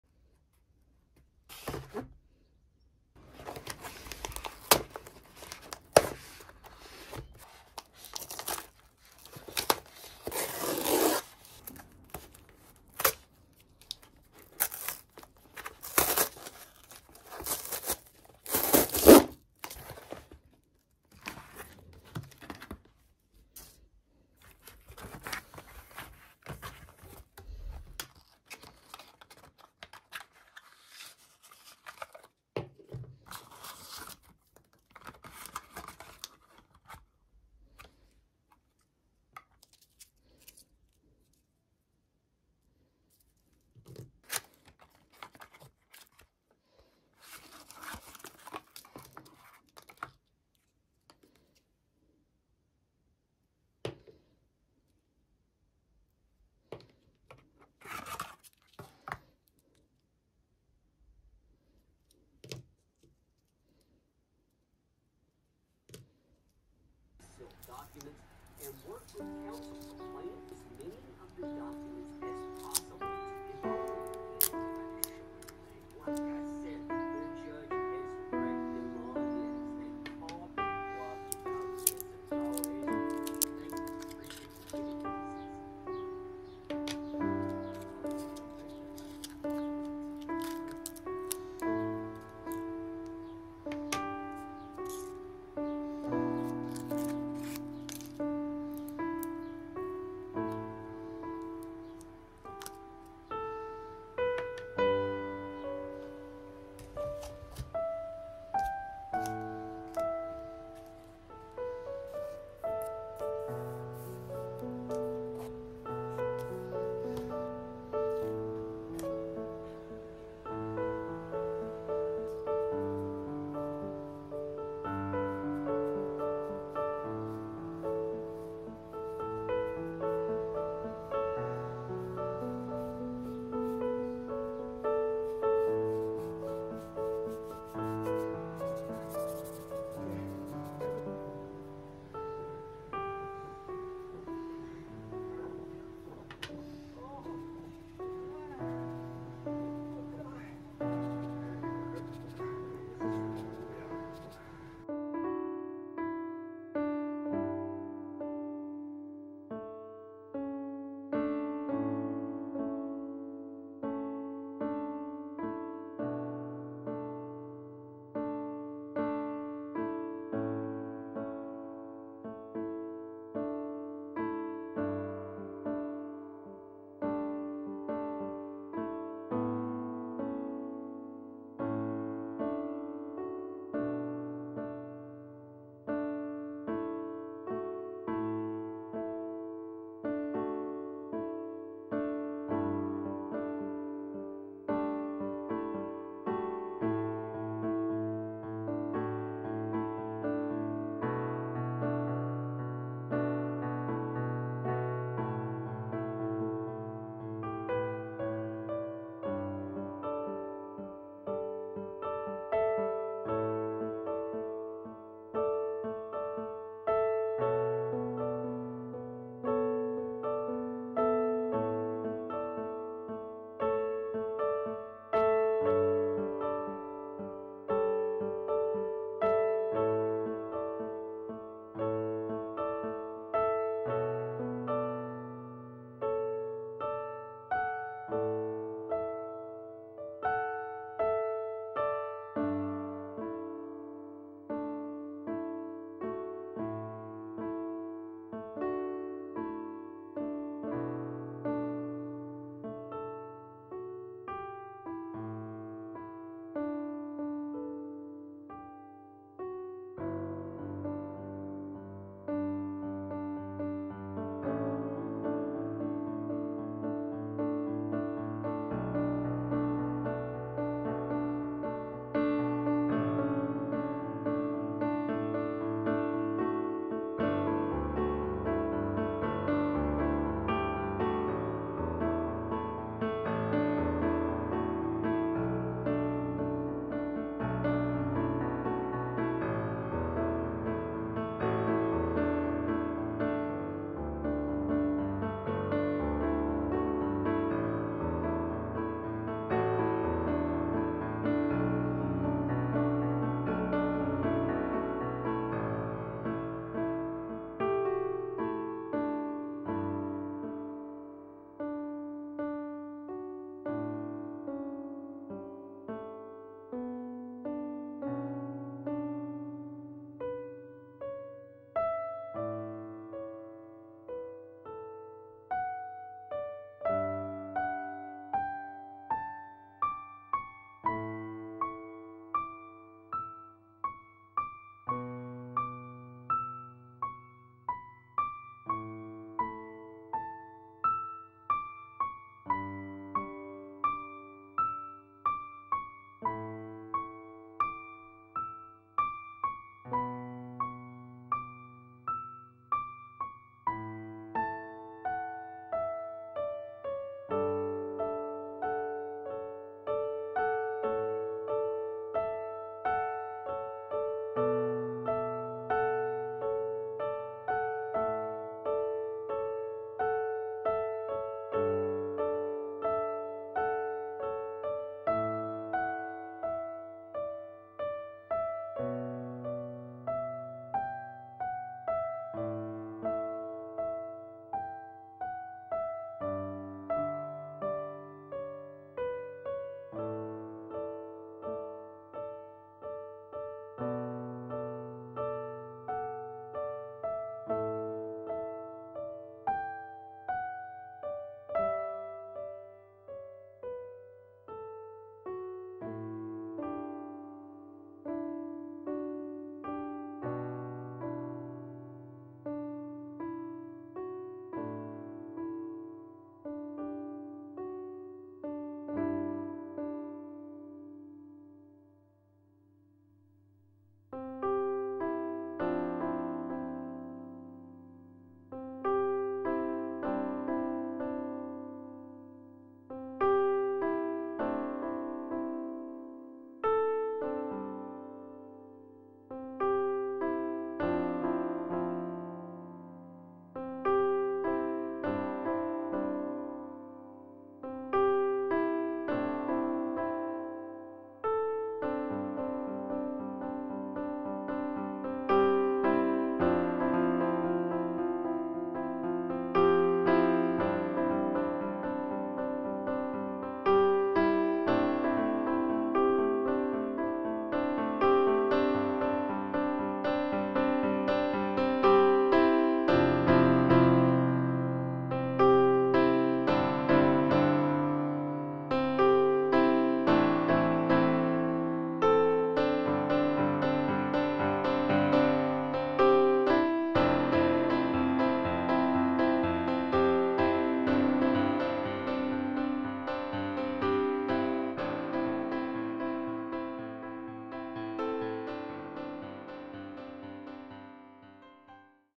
Mini MINI Art Haul - Roman Szmal And Turner Acryla Gouache
Heya all,

Skip to 5:04 to see the painting sketches with the watercolors if you don't wanna watch the swatching.

I treated myself to a few watercolors and acryla gouache. I can't really afford a big haul, but one day I plan to!

I love watercolors, I'm always adding things to wishlists and online baskets that never reach the checkout. There are so many things I want to try, and hopefully in time I can.

What paint do you love working with?

Paints used:
Roman Szmal: Quinacridone Scarlet, Mineral Violet, Paynes Grey and Aquarius Green

Turners acryla gouache: Violet, Aqua Green, Horizon Blue and Fresh Green